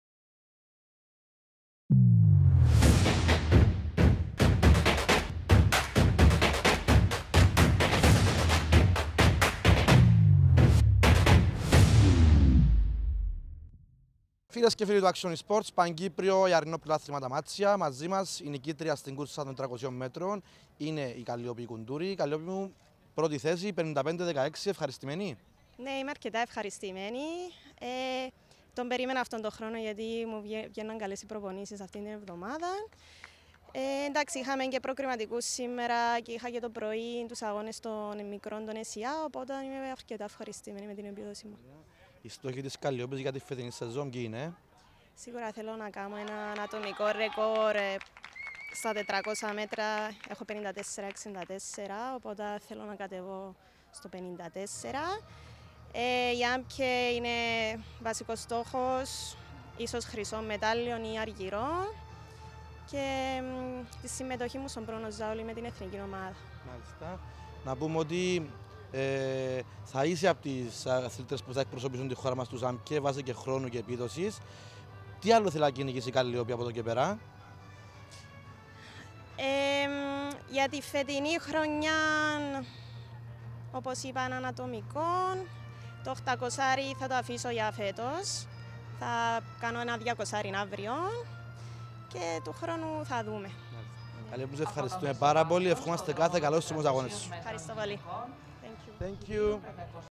Καλλιόπη Κουντούρη: «Στόχος το χρυσό ή αργυρό στους ΑΜΚΕ- Θέλω να κατεβώ στο 54’»
Welcome to the official ACTION IN SPORTS YouTube channel.
Find us on the internet:
Production by Action IN Sports
Reporter Paris J Andreou and Stelios Stylianou